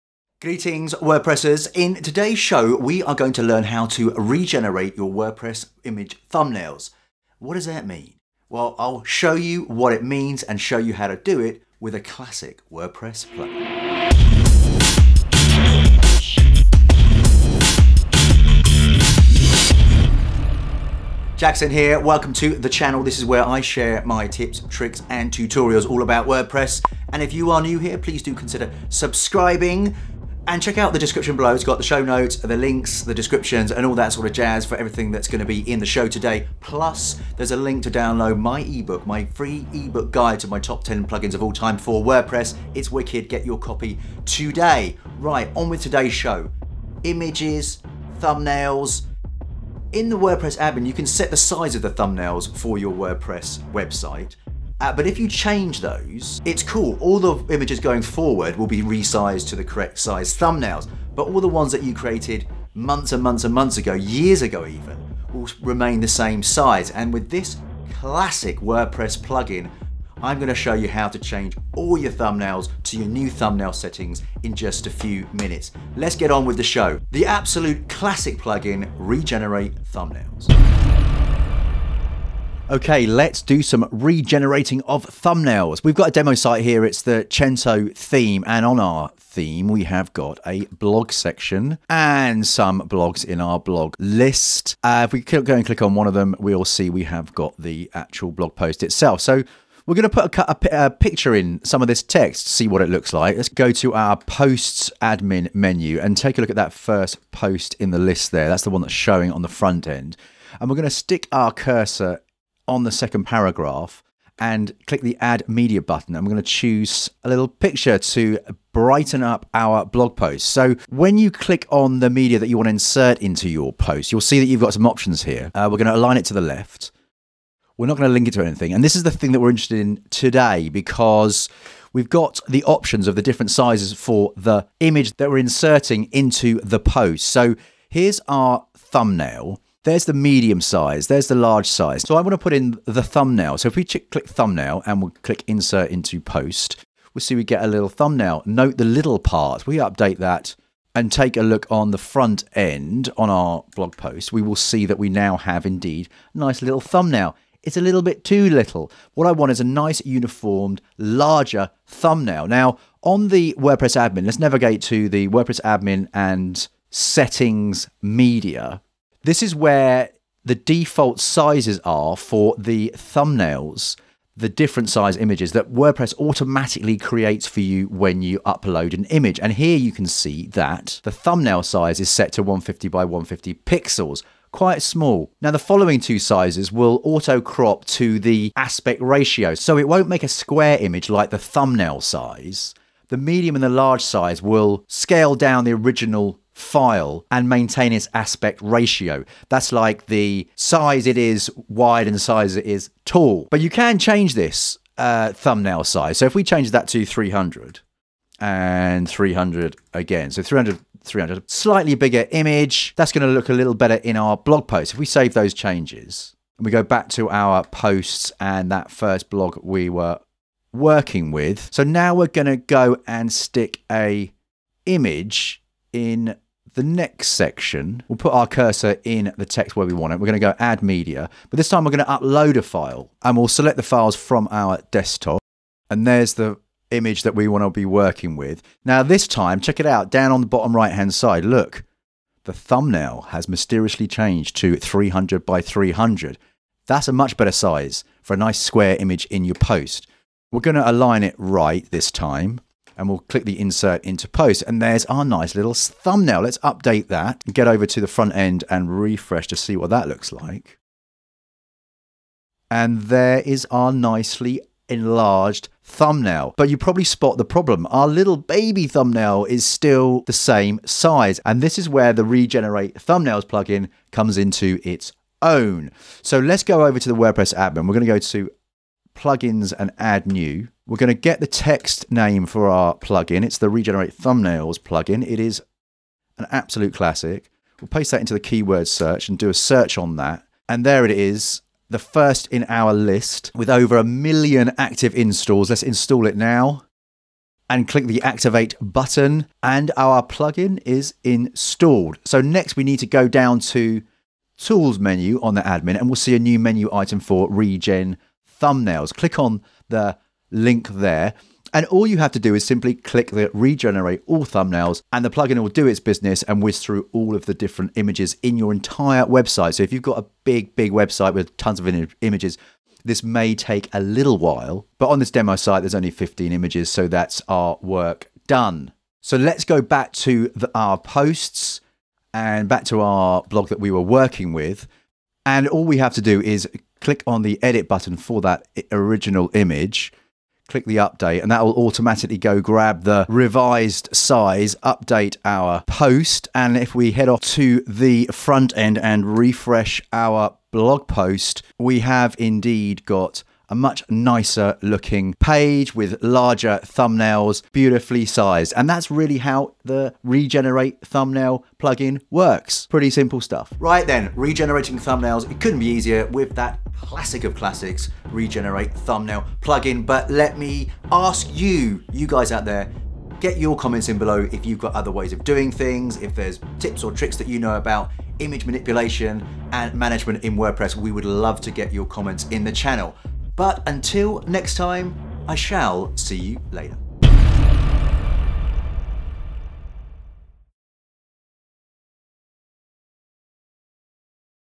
How To Regenerate Thumbnails In WordPress – WordPress Plugin Tutorial 2017
If you need to change the size of your WordPress Thumbnails it’s a complete breeze with the classic WordPress Plugin “Regenerate Thumbnails”.    ☆☆ ☆ Download my FREE 2019 Theme "Cento" - a WordPress Starter Theme for Elementor

In this WordPress Tutorial I’ll show you how to do it in just a few minutes.

The Plugin can be found on the WordPress Plugin directory

Lets Make some WordPress!
Jakson